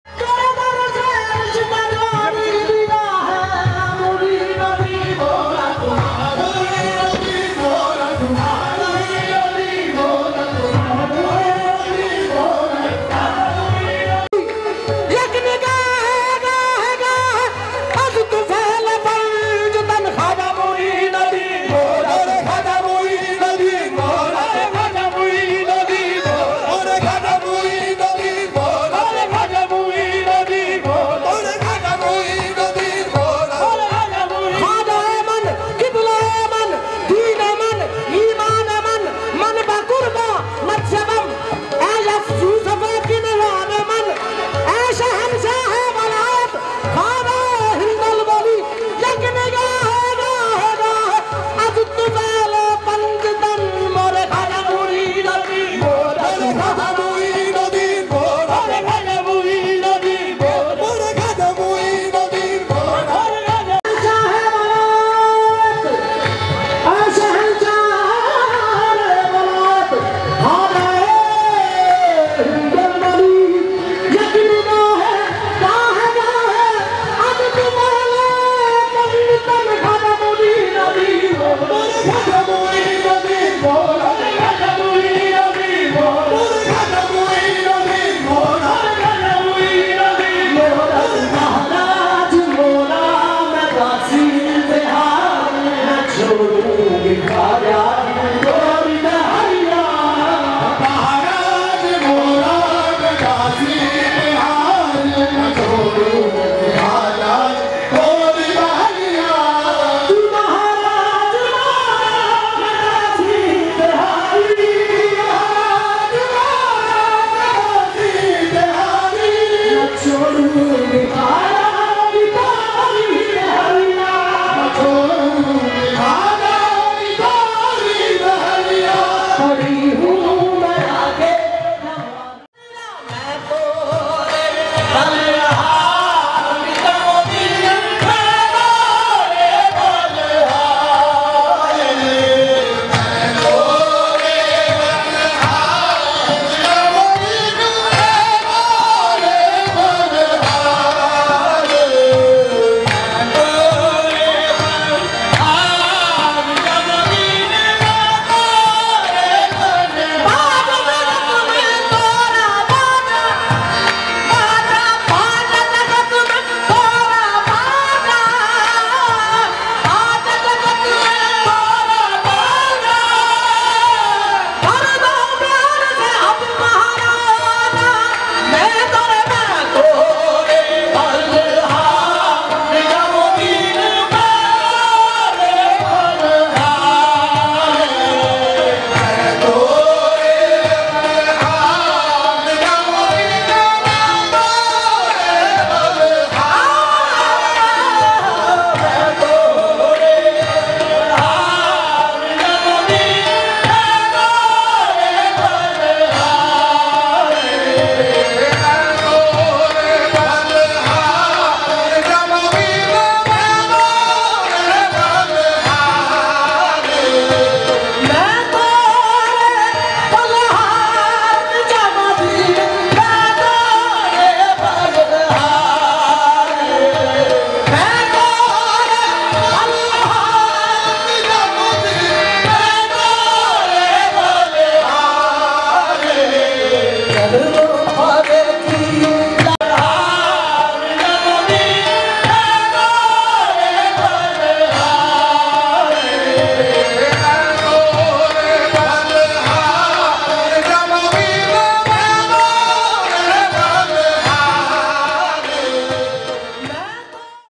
Wjd On Rahat Fateh Ali Khan Qawali |Hazrat Khawaja Tahir Nizami Sajada Nasheen| From India
Qawal- Rahat Fateh Ali Khan
Host -Khawaja Israr Ul haq Chishty Nizami Fakhri
Recording -Ahsan Jerry Studio
RahatFatehAli Khan